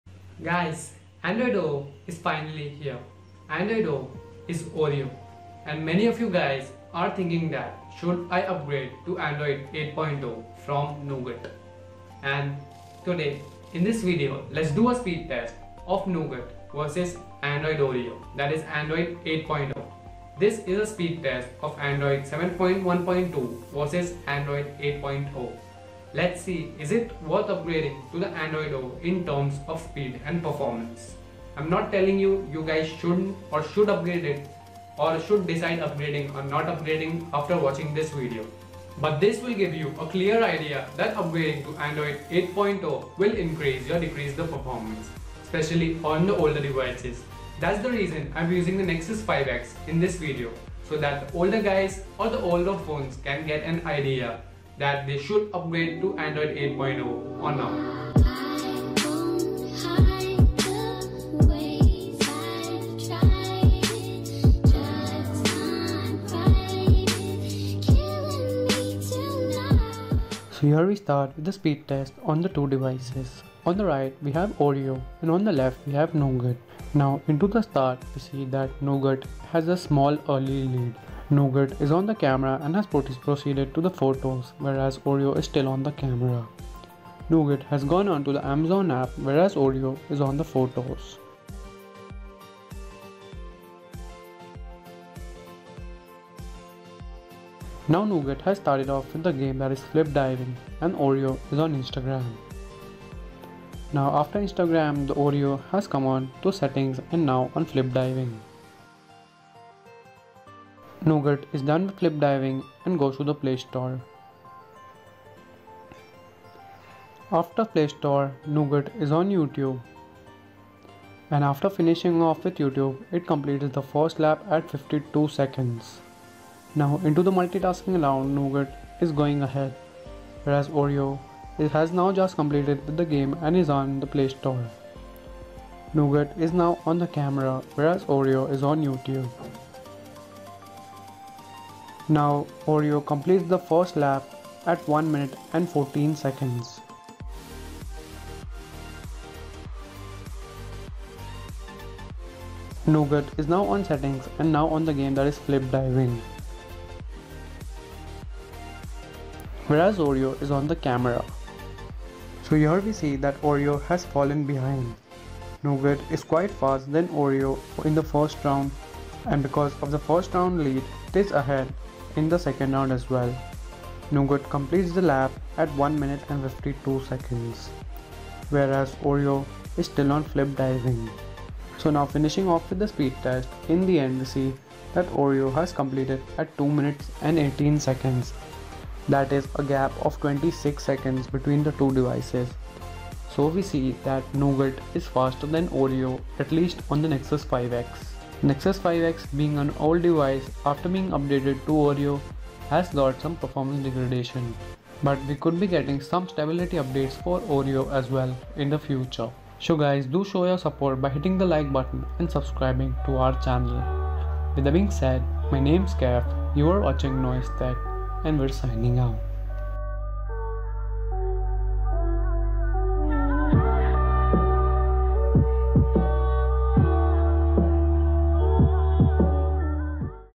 Android 8.0 Oreo vs Android 7.1.2 Nougat Ultimate SpeedTest!
Let's compare the new & greatest operating system Android 8.0 Oreo against the Android 7.1.2 Nougat in a speedtest battle. Let' see if it is worth upgrading to the new operating system